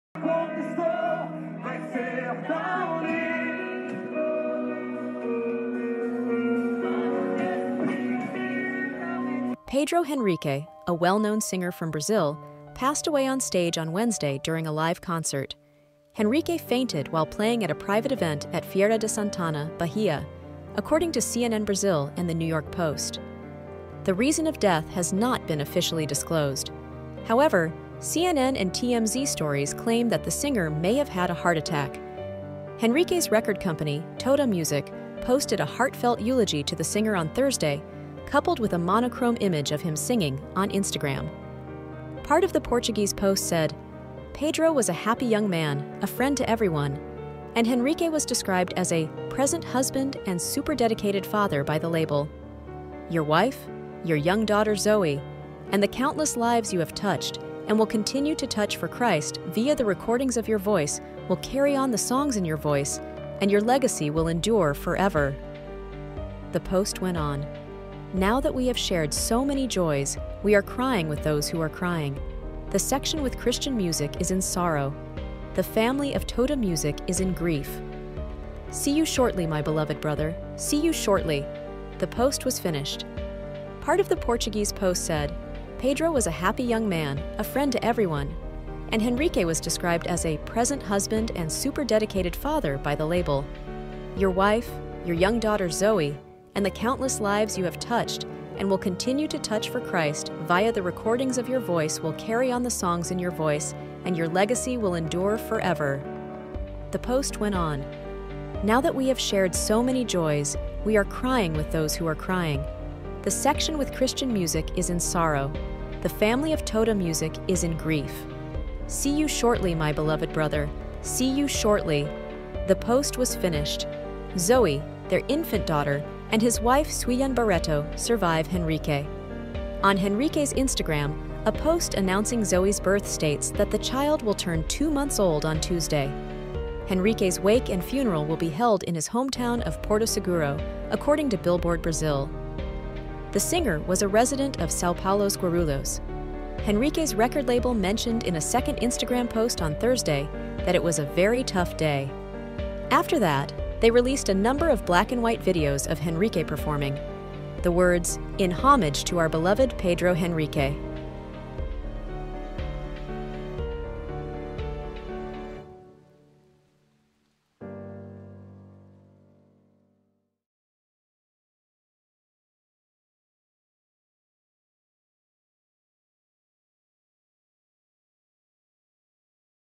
Pedro Henrique singer Brazilian Dead at 30 After Collapsing Onstage
Pedro Henrique, a well-known singer from Brazil, passed away on stage on Wednesday during a live concert.

Henrique fainted while playing at a private event at Fiera de Santana, Bahia, according to CNN Brazil and the New York Post.

The reason of death has not been officially disclosed. However, CNN and TMZ stories claim that the singer may have had a heart attack.

Henrique's record company, Todah Music, posted a heartfelt eulogy to the singer on Thursday, coupled with a monochrome image of him singing, on Instagram.

Part of the Portuguese post said, "Pedro was a happy young man, a friend to everyone," and Henrique was described as a "present husband and super dedicated father" by the label.

"Your wife, your young daughter Zoe, and the countless lives you have touched and will continue to touch for Christ via the recordings of your voice will carry on the songs in your voice and your legacy will endure forever!" The post went on.

"Now that we have shared so many joys, we are crying with those who are crying. The section with Christian music is in sorrow. The family of Todah Music is in grief.

"See you shortly, my beloved brother! See you shortly! The post was finished.

Zoe, their infant daughter, and his wife Suillan Barreto survive Henrique. On Henrique's Instagram, a post announcing Zoe's birth states that the child will turn two months old on Tuesday.

Henrique's wake and funeral will be held in his hometown of Porto Seguro, according to Billboard Brasil. The singer was a resident of São Paulo's Guarulhos.

Henrique's record label mentioned in a second Instagram post on Thursday that it was a "very tough day."
After that, they released a number of black-and-white videos of Henrique performing. The words "In homage to our beloved Pedro Henrique" were printed in Portuguese above the fold.
"We carefully considered whether it would be appropriate to share anything today," the post's Portuguese caption said. "We're mourning his passing and cherishing the precious times we spent with him—his infectious happiness, his mesmerizing voice, and his radiant smile."